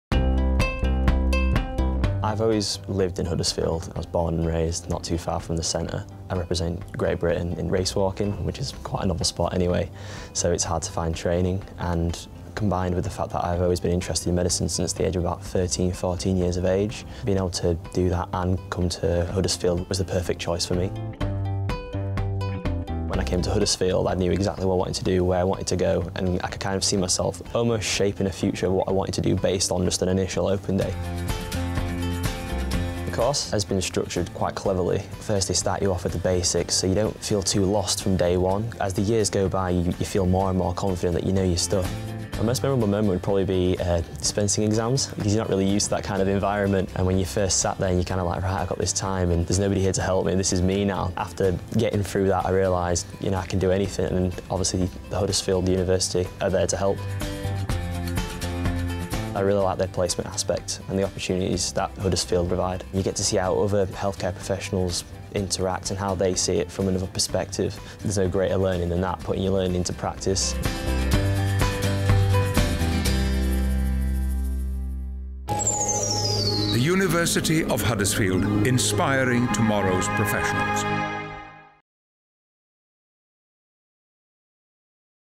Pharmacy at the University of Huddersfield - Jamie Higgins
Jamie is studying Pharmacy MPharm at the University of Huddersfield.

Watch his film to get an overview of the facilities available on the Huddersfield campus. Jamie also describes the opportunities his course provides for work placements and why he believes they are  one of the best ways to learn.